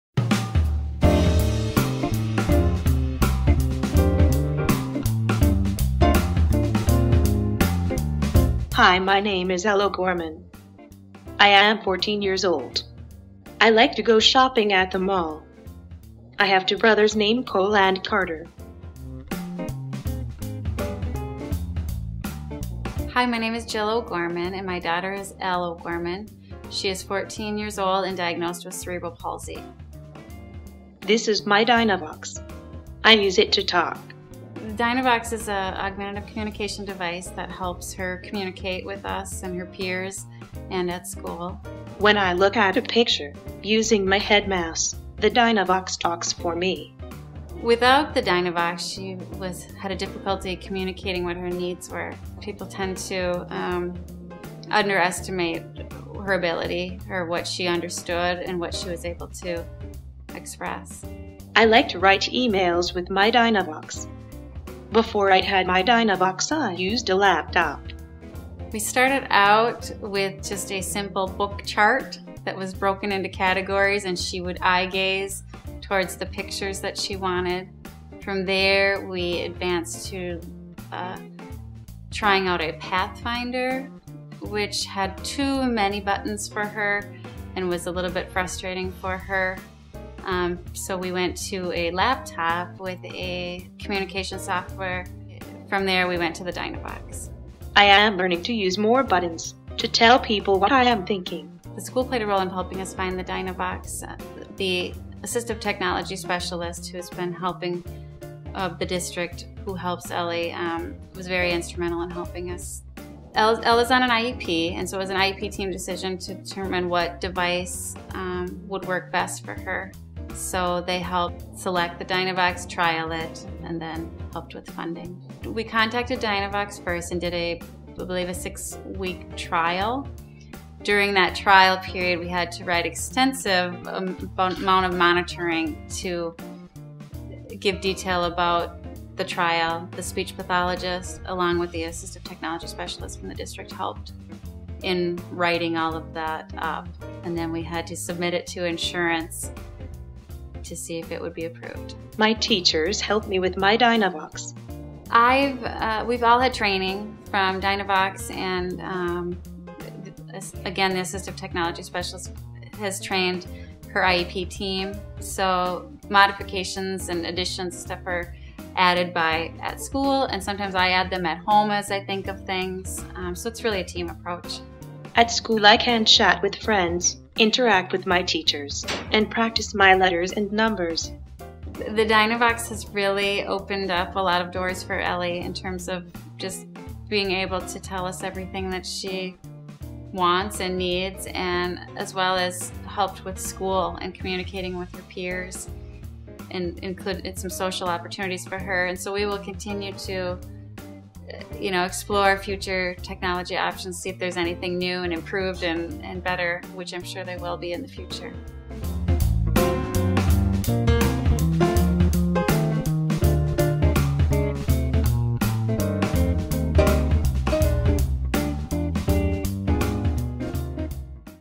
Assistive Technology in Action - Meet Elle
The Family Center on Technology and Disability (FCTD) and PACER Center are pleased to announce the release of our new assistive technology awareness series, AT in Action.  Funded by the U.S. Department of Education's Office of Special Education Programs (OSEP), this fully-captioned video series is designed to strengthen awareness of AT devices that help individuals with disabilities participate fully in school, at home, and in the community.  In this second video, you'll meet Elle O'Gorman, a young woman with cerebral palsy who, with the help of AT, is able to communicate with family, friends, and teachers.